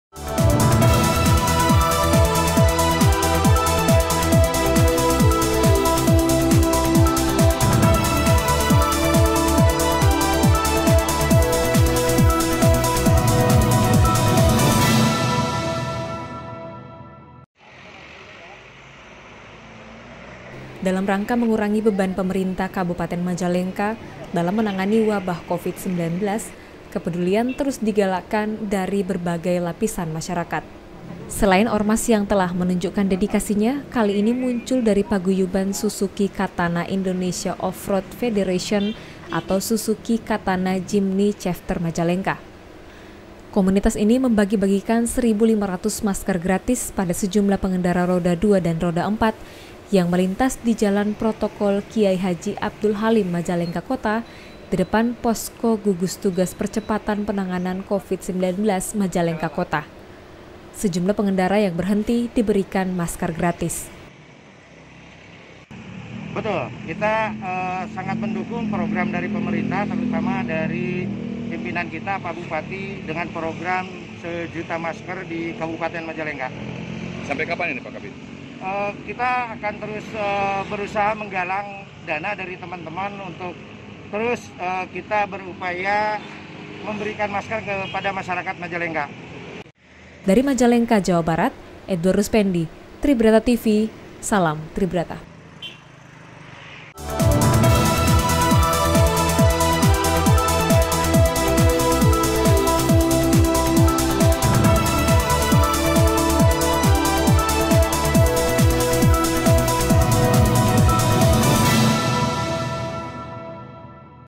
KOMUNITAS OFFROAD MAJALENGKA PEDULI WABAH COVID 19
Dalam rangka mengurangi beban pemerintah Kabupaten Majalengka dalam menangani wabah covid-19, kepedulian terus digalakan dari berbagai lapisan masyarakat.

Selain ormas yang telah menunjukan dedikasinya kali ini muncul dari paguyuban suzuki katana indonesia offroad federation atau suzuki katana jimny chafter Majalengka.

Komunitas ini membagi-bagikan 1500 masker gratis pada sejumlah pengendara roda dua dan empat yang melintas di jalan protokol kyai haji Abdul halim Majalengka kota di depan posko gugus tugas percepatan penanganan covid-19 Majalengka kota. Sejumlah pengendara yang berhenti diberikan masker gratis.